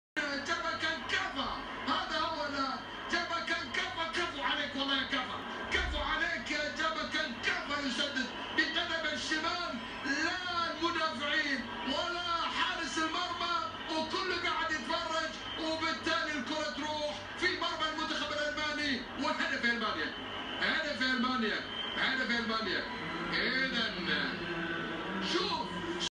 超ワールドクラスのボレー！ドイツ対ジョージア カンカバのゴール！
スアレスやサンチェス級のスーパーゴール！足は右に振り抜いているのに、アウトサイドでけっているのでボールはネット左側面に突き刺さる。ノイアー一歩も動けず